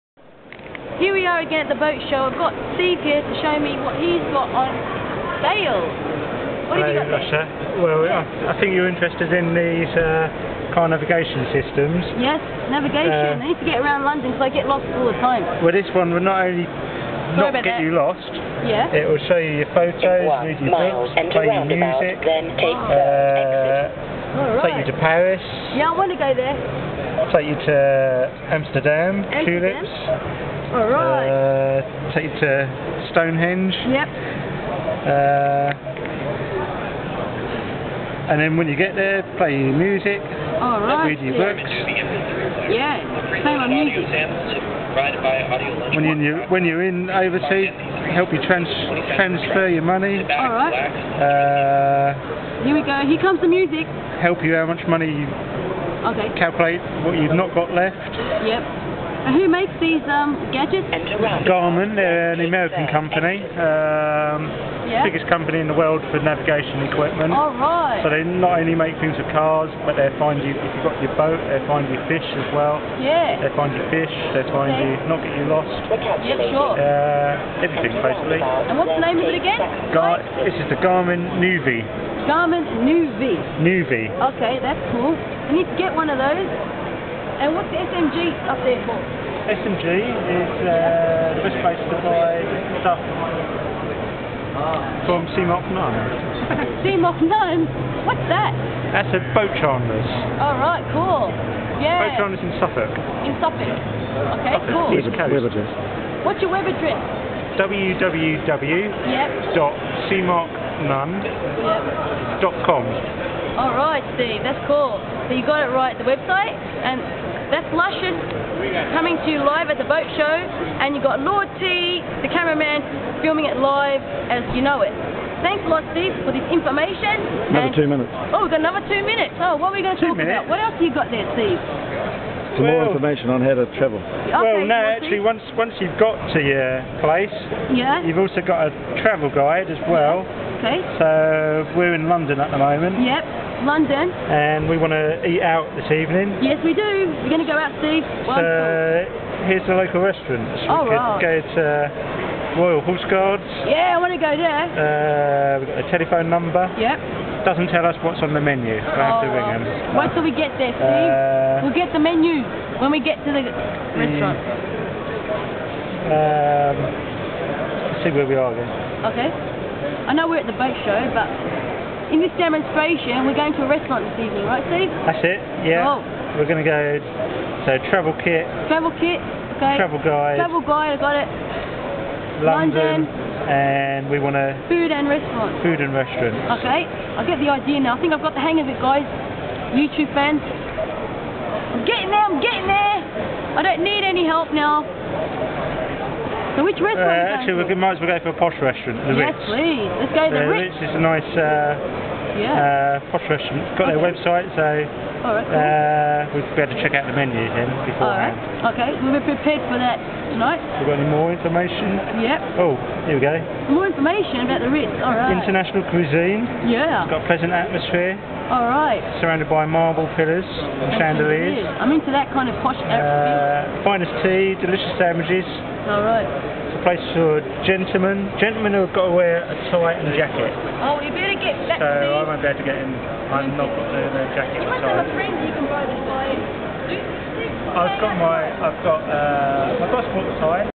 Steve talks with Topaz, at Garman
Steve talks to Topaz, at Garmin, London Boat Show